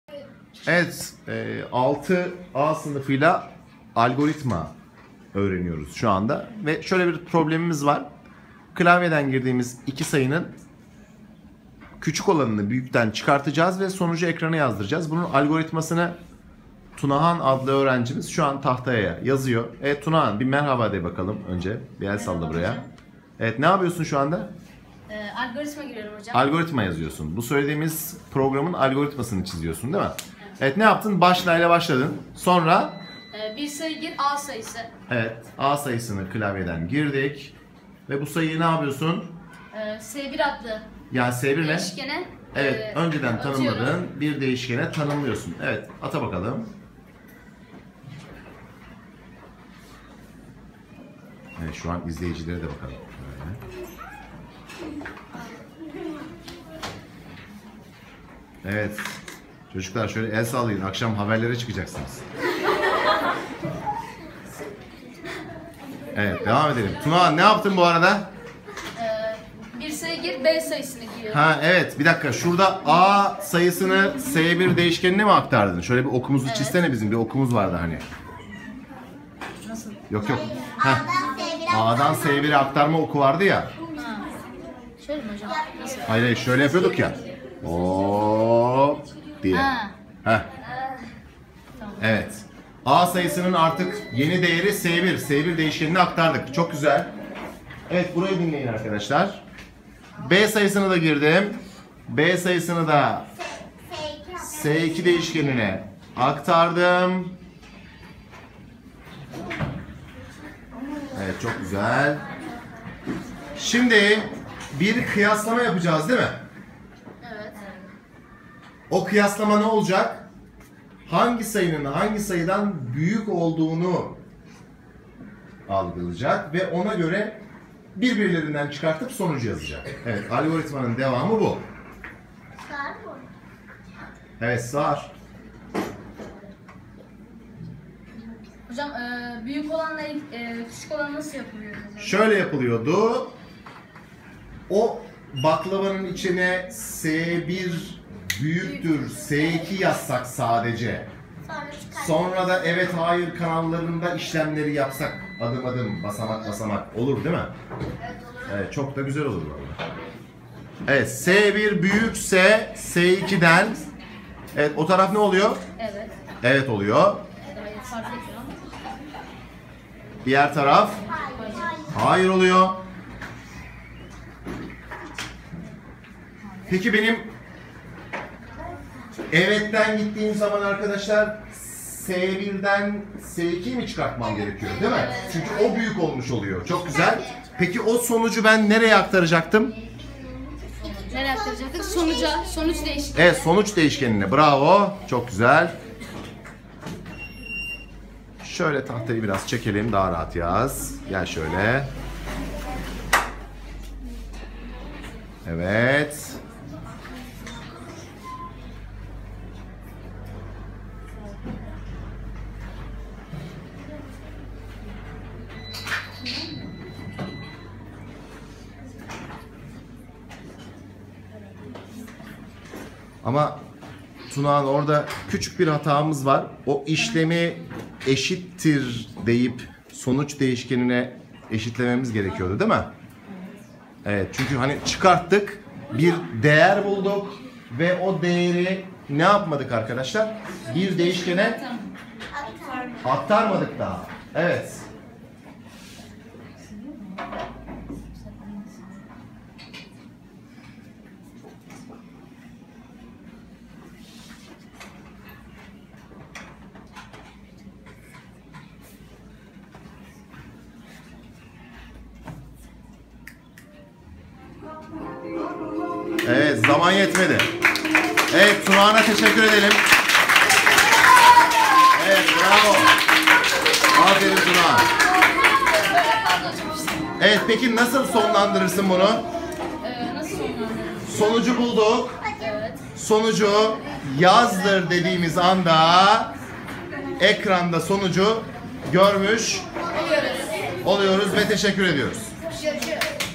Tunahan Cesur ile Algoritma
Yunusemre Çağatay Uluçay Ortaokulu 6-A sınıfı öğrencimiz Tunahan Cesur programlamanın ilk adımı olan algoritma yazıyor...